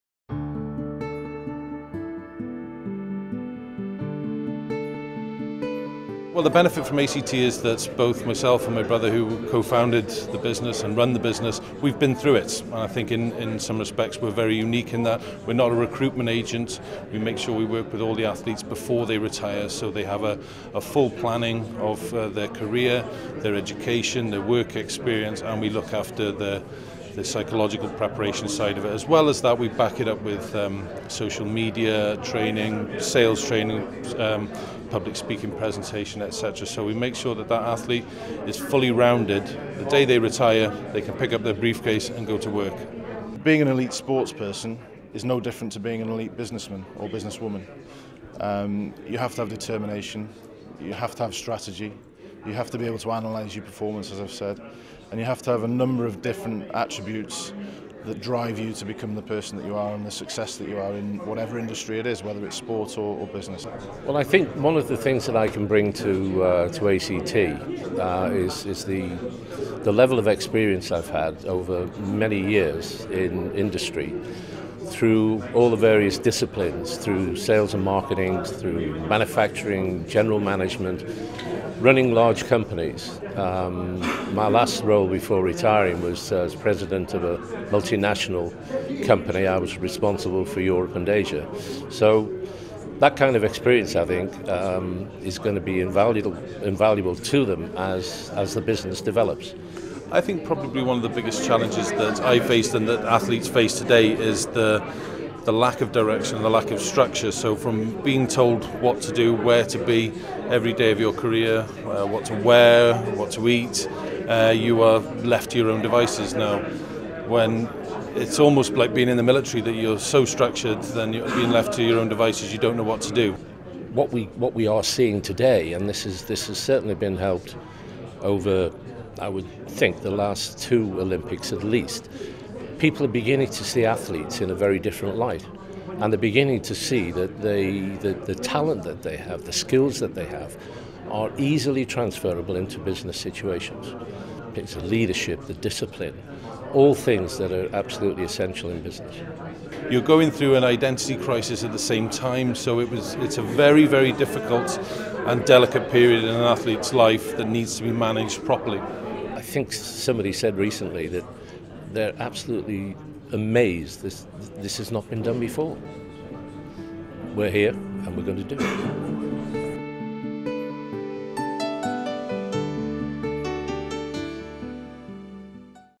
ACT Athlete Career Transition
In this video Andy and Steve Moore (Directors & Co-Founders of ACT) describe how as previous Wales Internationals they have been through the challenges of managing the transition from Elite Athlete to a new career and how they form ACT to help others.

They describe how they overcome the identity crisis and lack of structure that faces an athlete when they retire from elite sport.

George Moore (Chairman) also discusses how companies are starting to see elite athletes in a very different light.  The skills and talents they have are very transferable as outlined at the Leadership & Management event held in Cardiff, where this video was shot and at which 11 time Paralympian and ACT client Dai Roberts spoke. (see the other videos that accompany this one).